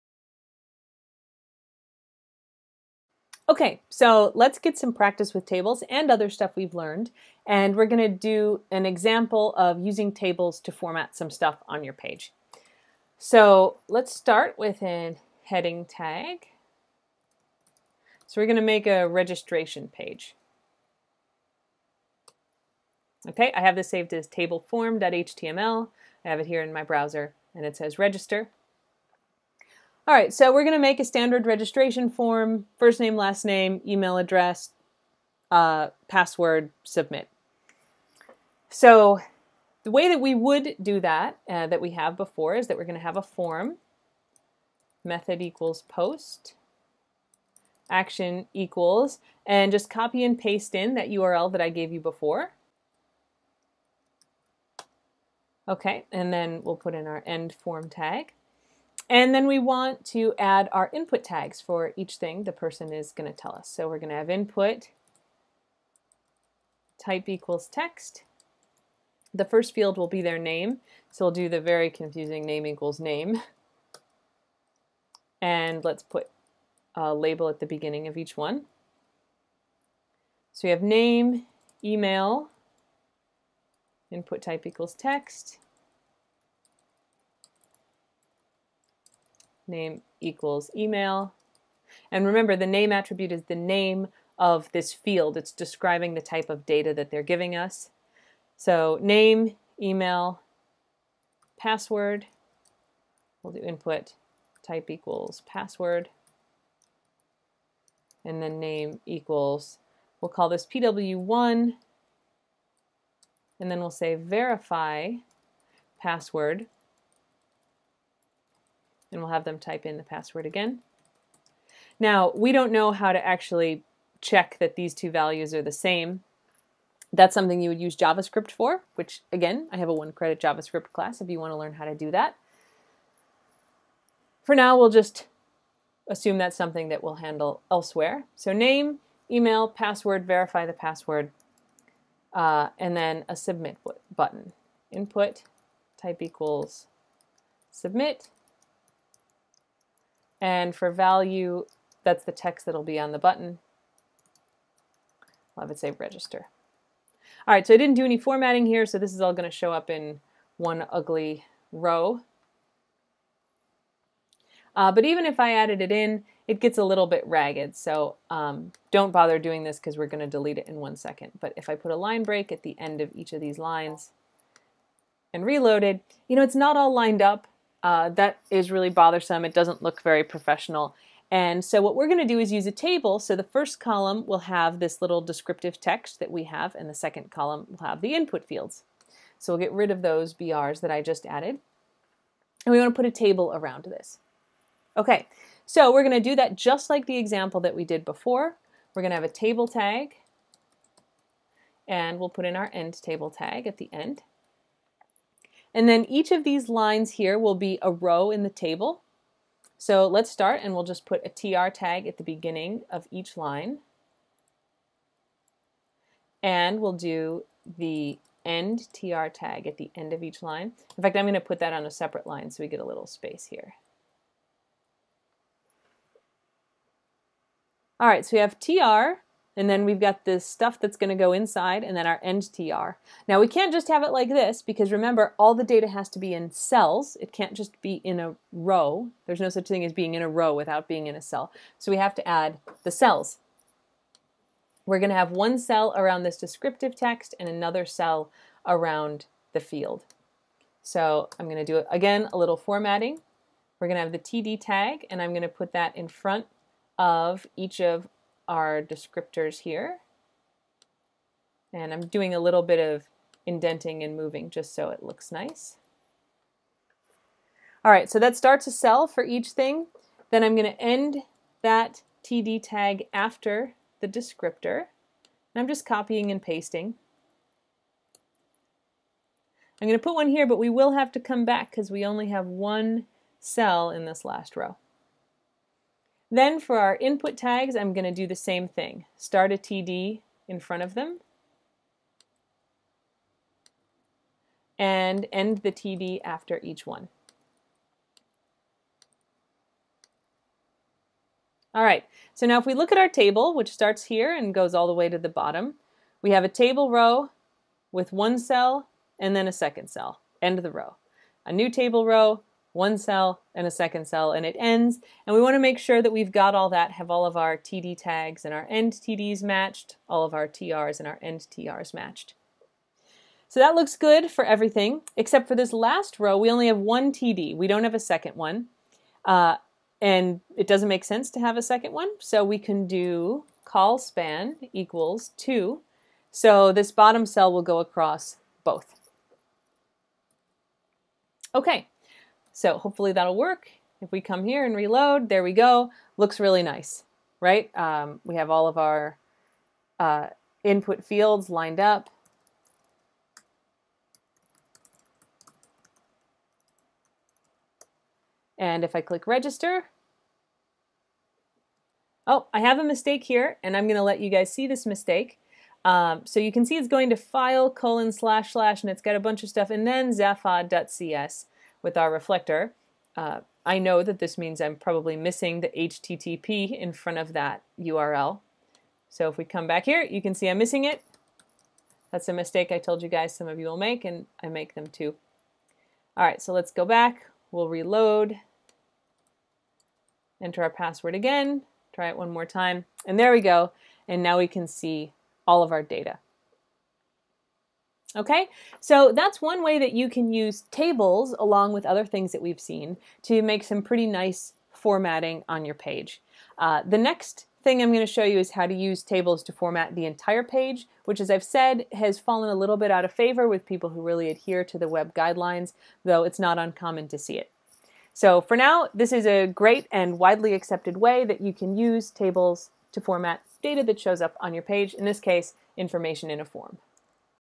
Formatting Forms with Tables in HTML
How to use tables to format forms in html